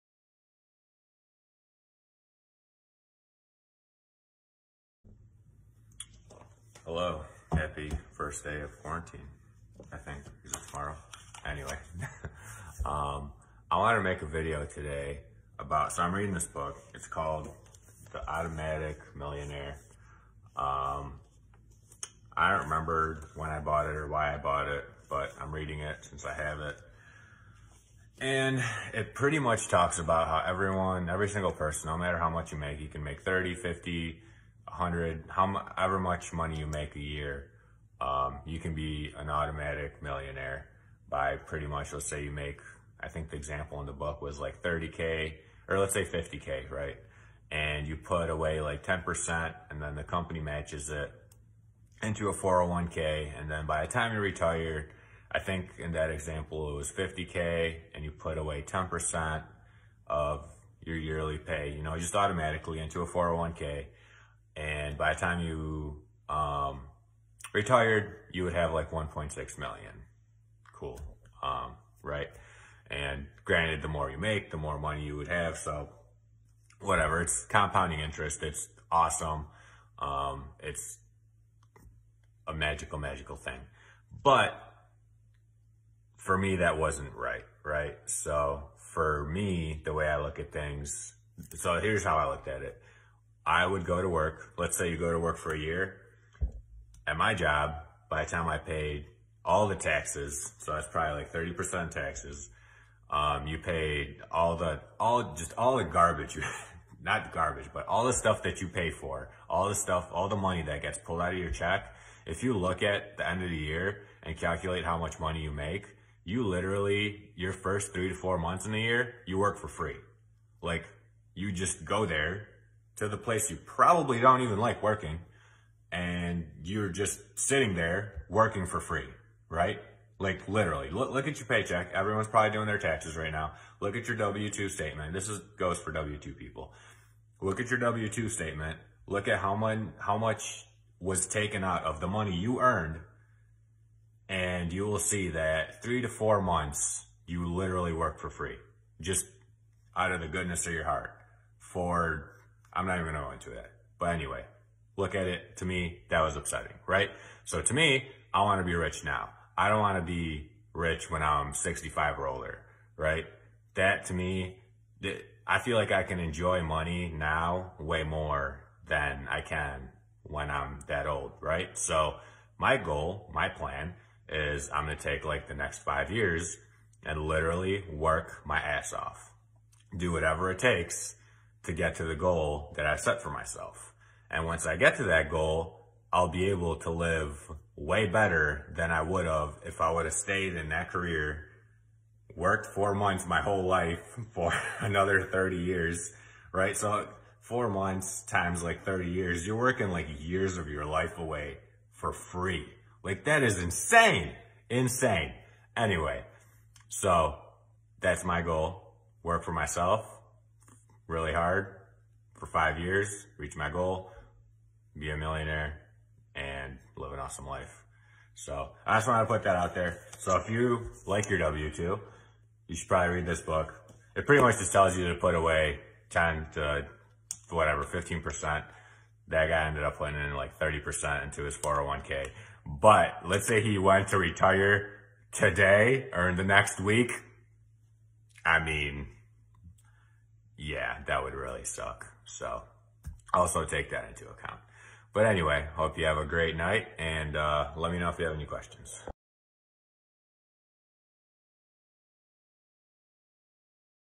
Do You Want To Be An Automatic Millionaire?
Do you really want to wait to be rich when you are 65 or older? Or would you rather work your butt off for the next 5 years to be rich in 5 years? Reading the book Automatic Millionaire brought up some thoughts and reasons as to why I left my old job, I don't want to wait that long to enjoy my life. This episode is a throwback to earlier in the year, but it still applies.